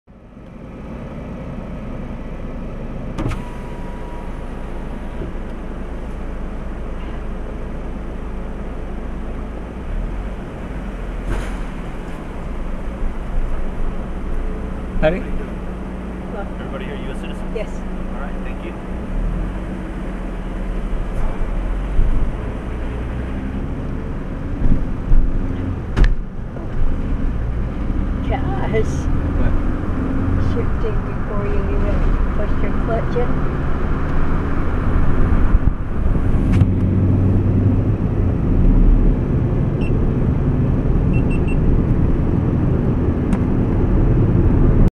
Everybody here a US Citizen? Border Patrol's Inland Immigration Checkpoint, 10 July 2018
Everybody here a US Citizen? Border Patrol's Inland Immigration Checkpoint, 10 July 2018, Ajo-Gila Bend Highway, Arizona State Route 85, GOPR1993